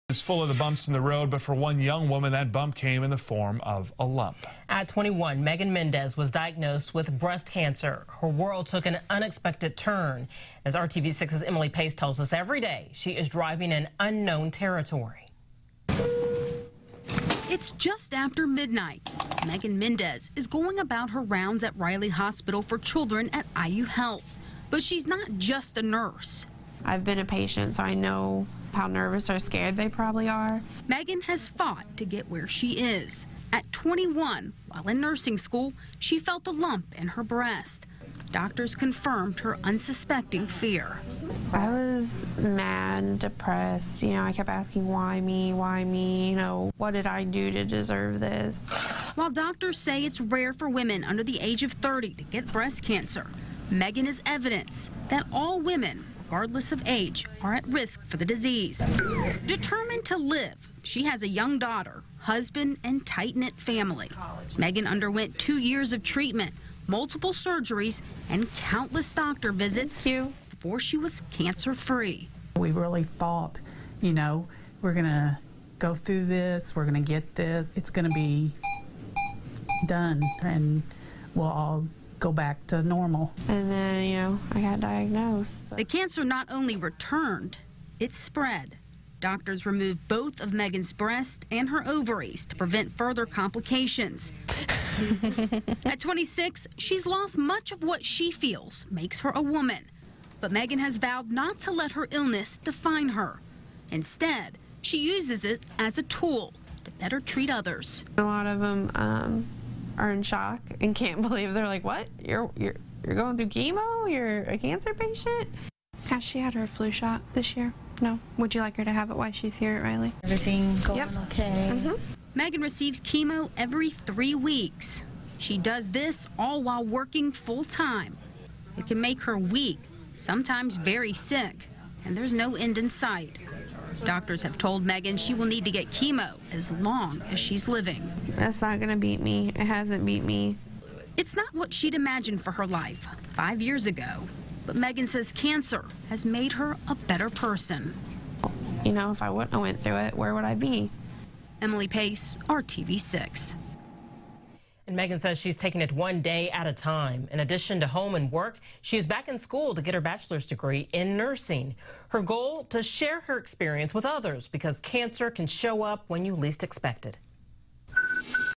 21-year-old nurse diagnosed with breast cancer
The working mom receives chemo every three weeks